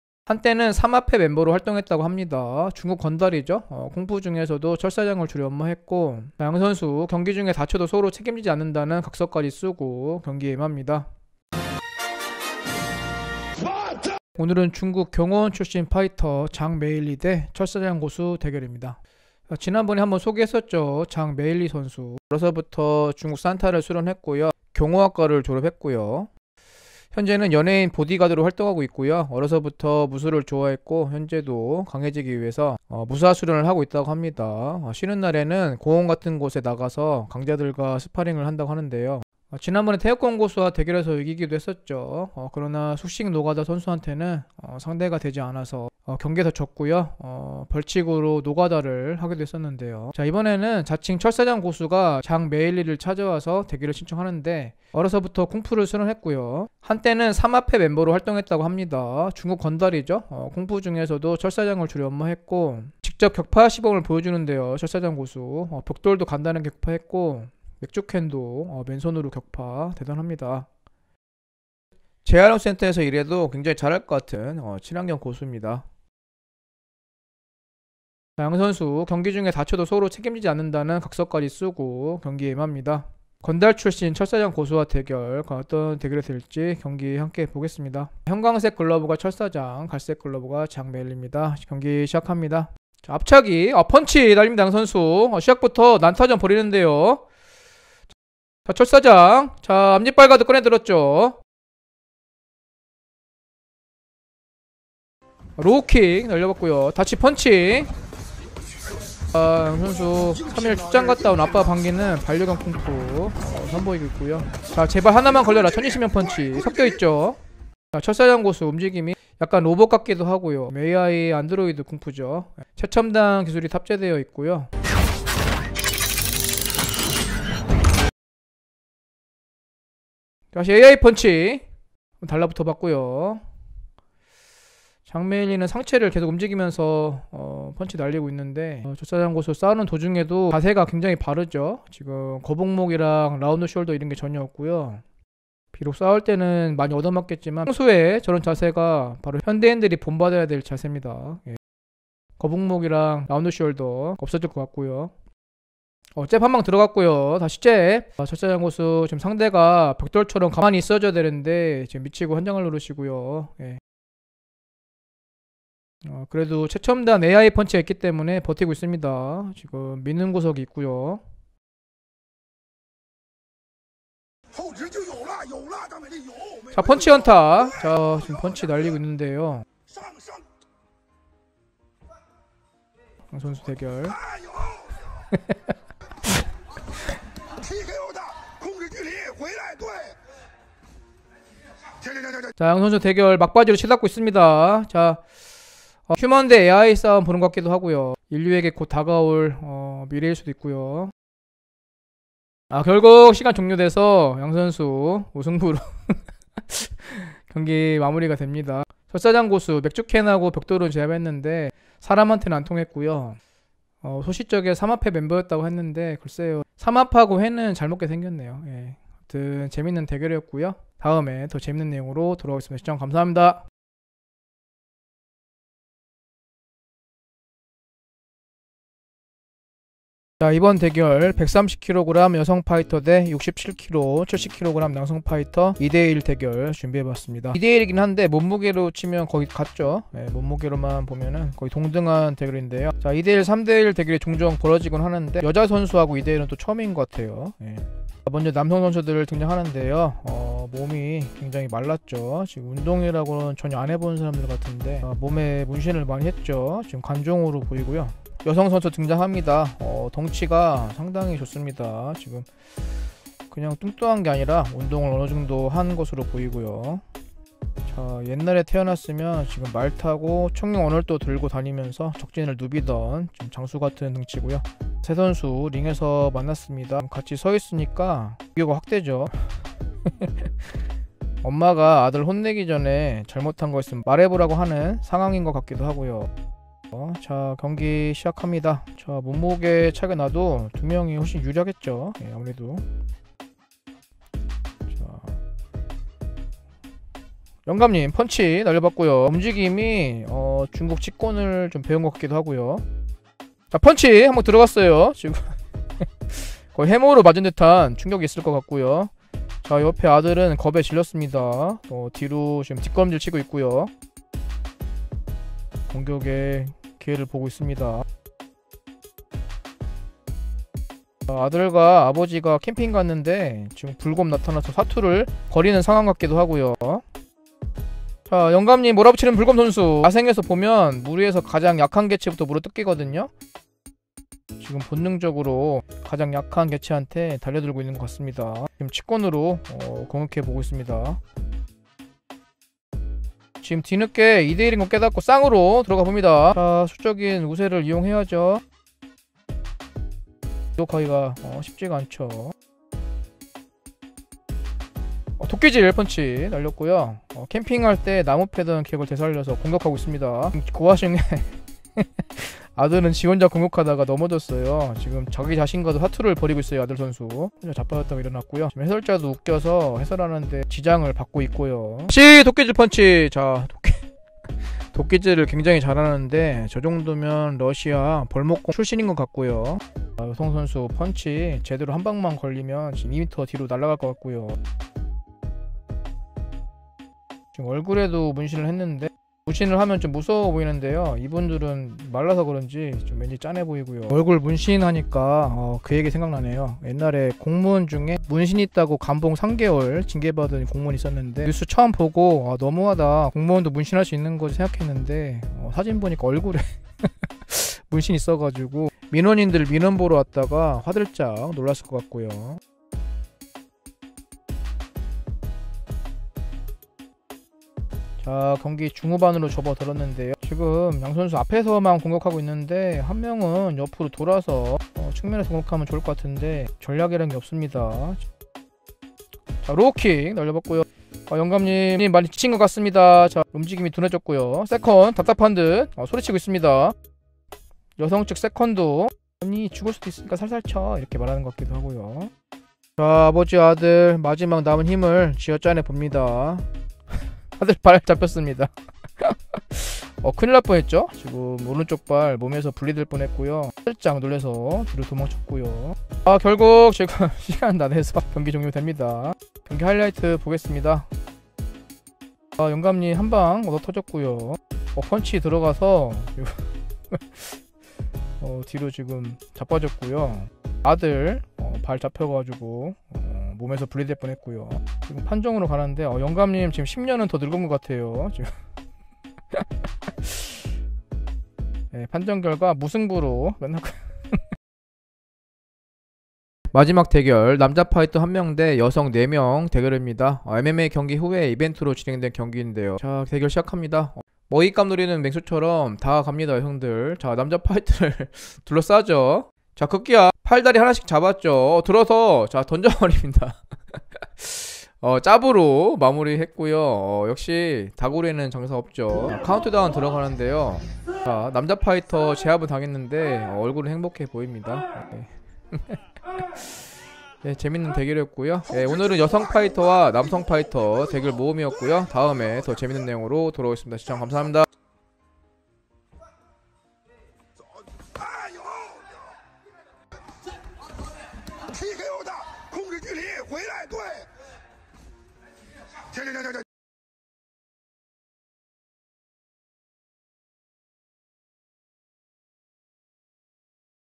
중국 삼합회 출신 철사장 고수 쿨제이?? vs 여자 경호원 대결, 4:1 남녀 대결까지 [한결TV]
안녕하세요 한결TV입니다.
자칭 삼합회 건달 출신 철사장 고수가 여자 경호원 파이터 장메일리와
대결을 신청하는데요 과연 결과는??
2:1 남녀 대결과 4:1 남여 대결까지 재미있는 결과네요 ㅎ

'페이스북 등에 무단 배포 금지'

'모든 영상 해설에는 한결tv만의 비평이 포함되어 있습니다.